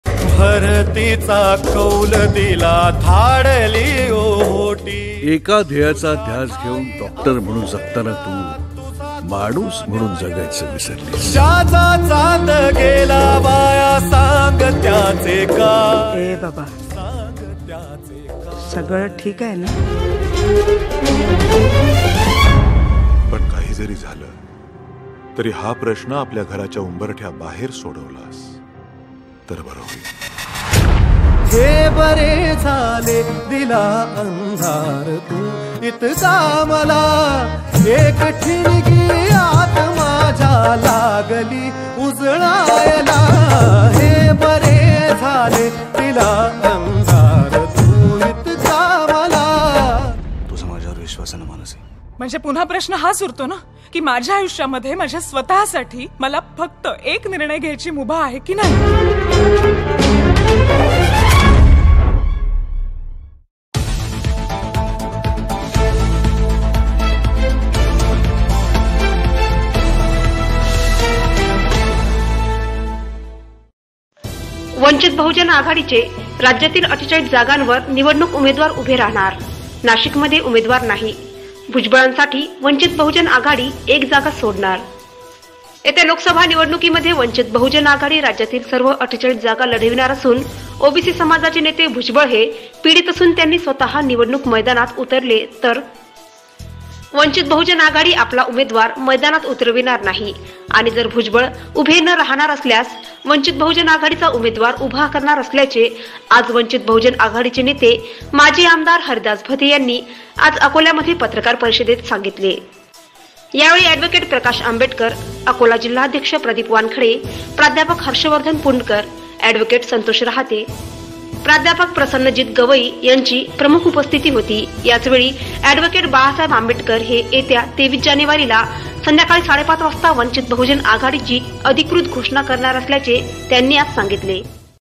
BREAKING-राज्यातील 48 जागांवर "वंचित बहुजन आघाडी" चे उमेदवार...
वंचित बहुजन आघाडी राज्यातील 48 जागांवर निवडणूक उमेदवार उभे राहणार ...नाशिक मध्ये उमेदवार नाही... भुजबळ साठी वंचित बहुजन आघाडी १ जागा सोडणार.

येत्या लोकसभा निवडणुकीत  वंचित बहुजन आघाडी  राज्यातील सर्व 48 जागा लढविणार असून ओ बी सी समाजाचे नेते भुजबळ  हे पीडित असून त्यांनी स्वतः निवडणूक   मैदानात उतरले तर वंचित बहुजन आघाडी  आपला उमेदवार  मैदानात उतरविणार नाही आणि जर भुजबळ उभे न राहणार असल्यास  वंचित बहुजन आघाडी चा उमेदवार  उभा करणार असल्याचे आज वंचित बहुजन आघाडीचे नेते माजी आमदार  हरिदास  भदे यांनी आज अकोल्यात पत्रकार परिषदेत   सांगितले यावेळी एड प्रकाश आंबेडकर, अकोला जिल्हाध्यक्ष  प्रदीप वानखडे, प्रा हर्षवर्धन पुंडकर, एड संतोष राहते, प्रा प्रसन्नजित गवई यांची प्रमुख उपस्थिती होती याचवेळी एड बाळासाहेब आंबेडकर हे येत्या 23 जानेवारीला संध्याकाळी 5 - 30 वाजता वंचित बहुजन आघाडीची अधिकृत घोषणा करणार असल्याचे त्यांनी आज सांगितले